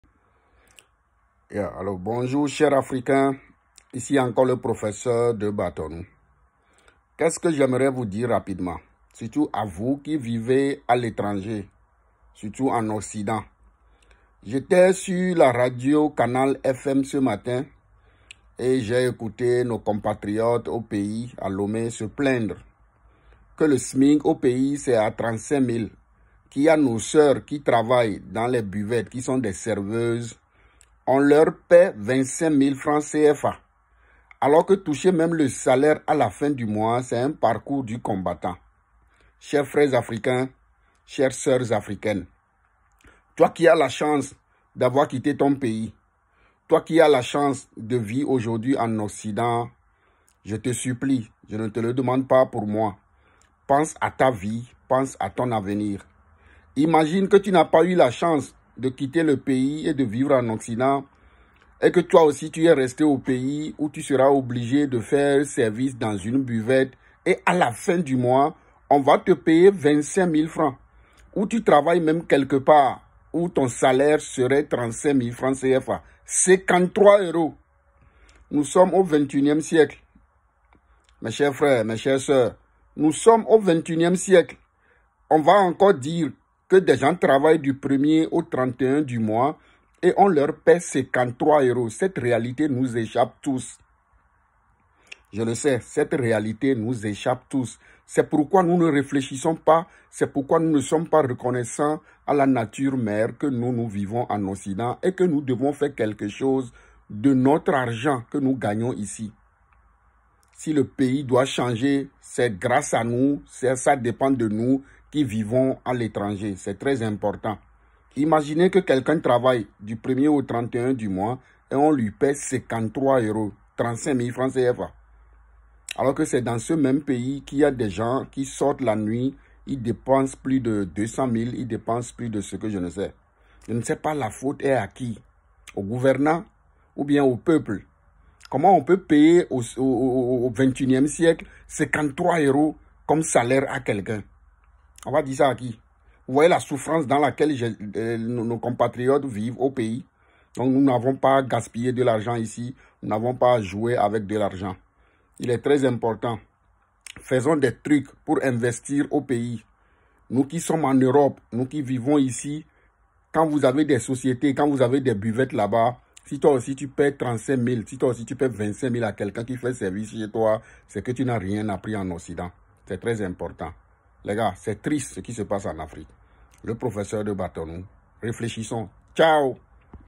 Le Professeur de Batonou te rappelle la misère au pays pour bien gérer ton argent gagné en Occident
Si tu as la chance d'avoir quitté le pays où 35 mille frcs CFA est le salaire à la fin du mois des millions de compatriotes, alors redeviens réaliste et conscient en investissant bien ton argent que tu gagnes en Occident.

Les choses vont encore devenir plus dures dans les années à venir et la misère envahira la vie de milliards d'humains.

Peu importe qui tu es, où tu vis et ce que tu possèdes, investis bien tes économies et arrête de gaspiller de l'argent peu importe comment tu le gagnes.

Il est grandement temps que nous pensions à notre retraite et à nos années de vieillesse surtout nous qui vivons à l'étranger. L'argent est comme un étranger, il finit des fois par partir en surprise tout comme il est arrivé.

Je t'invite à devenir réaliste et conscient et de refuser d'avoir honte demain et te plonger dans la souffrance malgré que tu aies passé de nombreuses années en Occident.

Réveillons-nous et investissons dans l'immobilier et terrains agricoles au pays pour notre avenir et celui de nos enfants...

Le Professeur de Batonou.